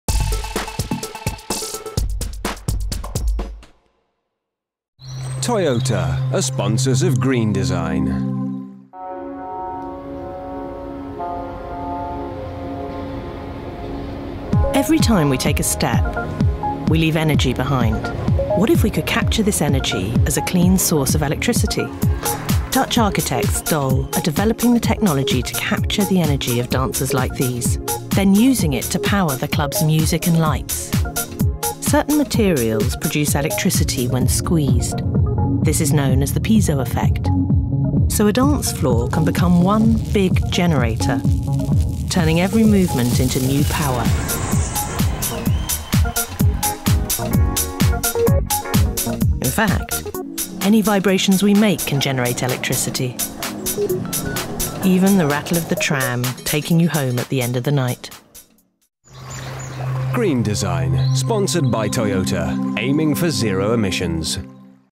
Dutch nightclub using energy from dancers to power lights, m
A short clip from green.tv highlighting a Dutch initiative at producing a kinetic energy capture system that can convert the energy imparted by people dancing into electricity to power the lights, music, etc.